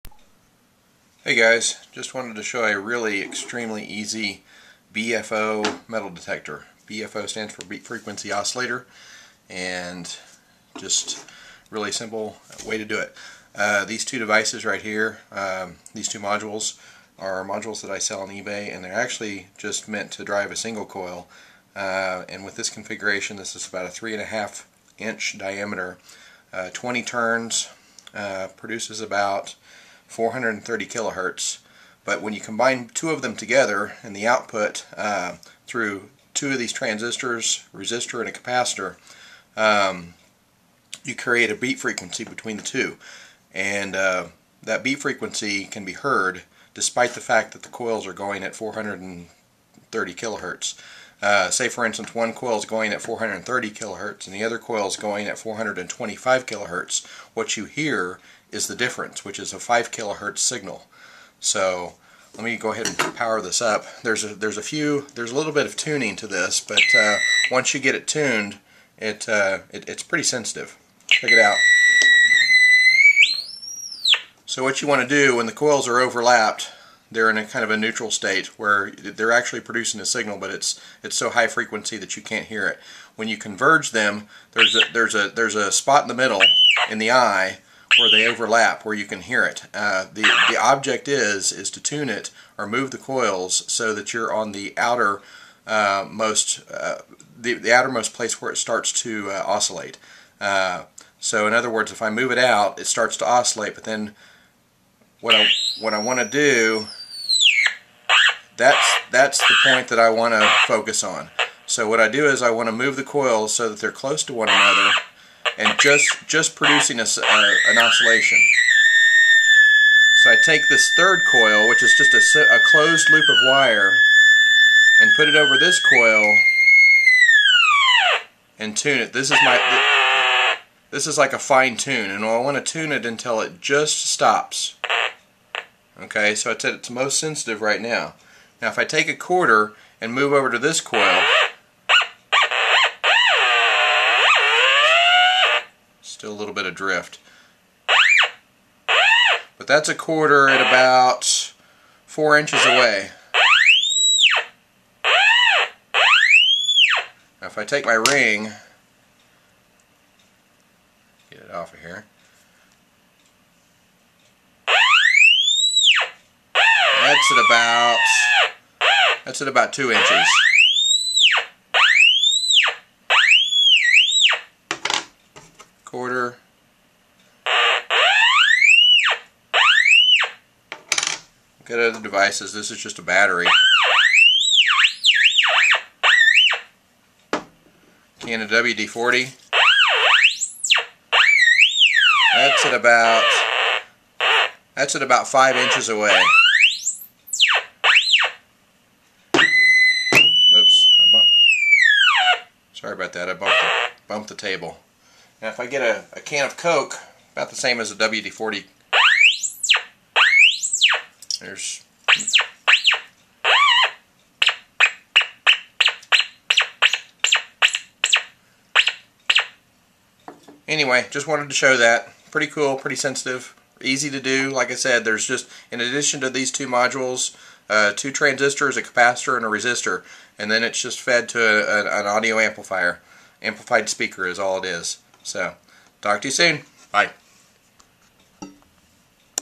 Simple BFO Metal Detector - Improved Circuit Design
eBay link (schematics included)

This is an extremely simple BFO style metal detector demonstration using two basic coil / inductor oscillators running at about 430kHz producing an audible beat frequency in the presence of metal objects.

How it works:
This particular arrangement "looks" like a typical Double D style coil arrangement, but don't let it fool you.  A "DD" has one drive coil and one receive coil.  In this case both coils are being driven.  When two coils that are driven share the same plane they will mutually oscillate and seek the same frequency.  The magic happens when the coils overlap slightly.  When they overlap at a certain point there is a balance where the mutual oscillation goes away and they don't influence each other, allowing them to freely oscillate and become easily altered by their environment ... i.e. a metal object.

  The Idea is to place the two coils in this overlapping arrangement so that they are JUST on the edge of influencing each other at the same oscillation, but are very susceptible to being altered externally.  This type of detector has a slight advantage over a standard BFO in that "both" coils are near the sense field which help to minimize any ground effects you might otherwise have using a single coil near the sense field.  Ground effects are caused by soil variations, such as mineral content, moisture, etc. Since both coils experience virtually the same ground conditions (unless passing over a metallic object), ground effects are mathematically canceled out leaving you with a higher signal to noise ratio when detecting.